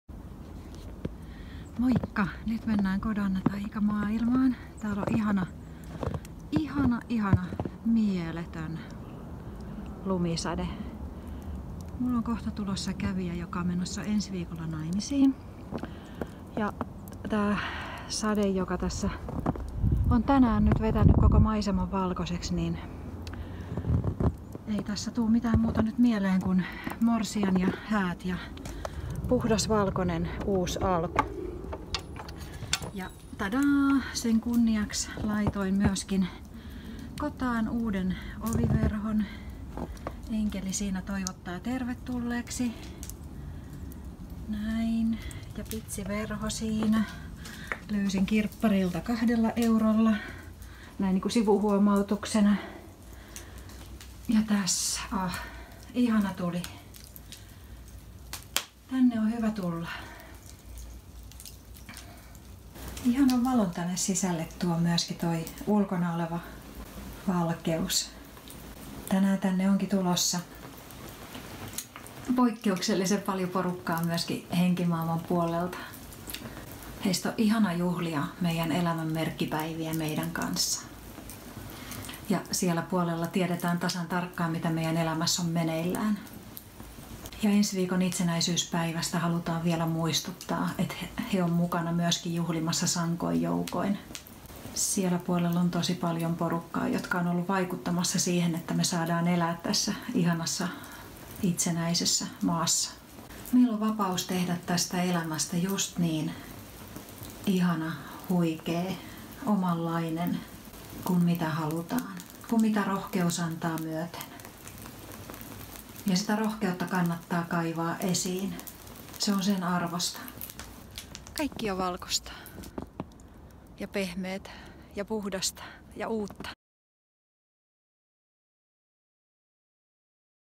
🙏Elä elämääsi rohkeasti
Tämä video syntyi Suomi 100 -juhlallisuuksien tiimellyksessä. Vaikka video on pikkuinen sen viesti on suuri: Elä elämääsi rohkeasti ja pelotta.

Lisää aiheesta löydät Valo minussa -podcastin jaksoista. Yli 50 jaksoa tietoisempaan elämään & henkiseen kasvuun